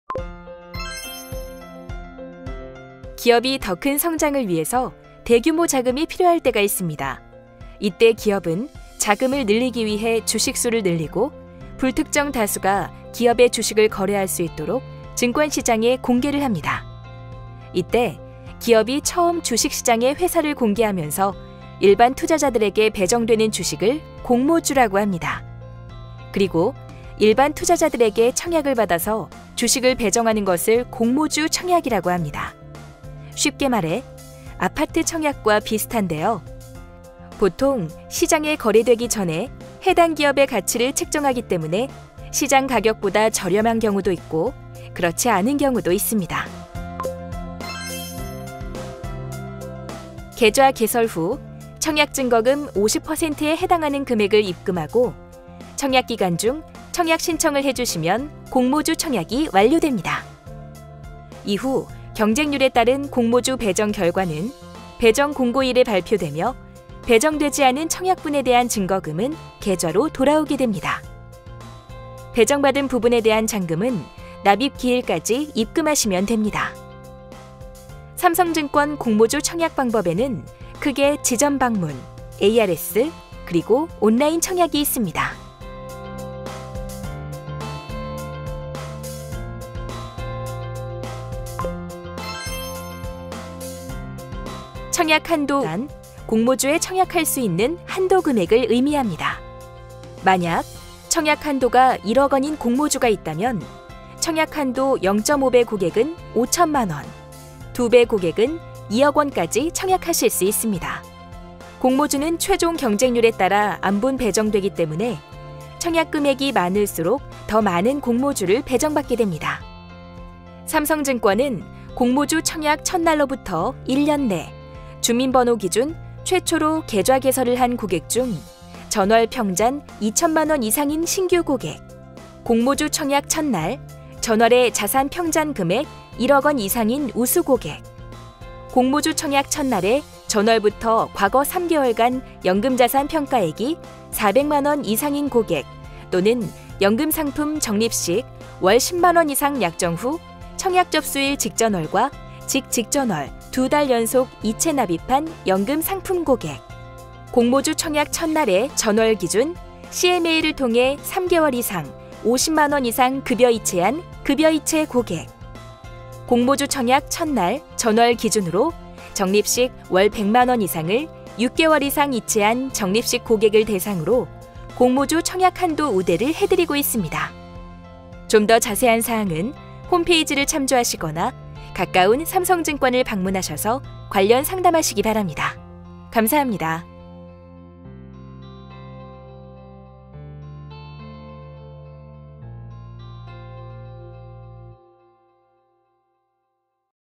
공모주 청약 안내
공모주? 공모주가 뭔지 궁금하셨죠?
공모주란 기업이 처음 주식 시장에 회사를 공개하며 투자자들에게 배정하는 주식~!!
이 주식을 신청하는 일련의 활동을 공모주 청약이라고 합니다.
삼성증권과 함께 주식시장에 데뷔하는 기업의 주주가 되는 법,
함께 알아보실까요?

00:00  공모주 청약이란 뭔가요?
00:47 공모주 청약 방법은?
01:32 공모주 우대 자격 조건은?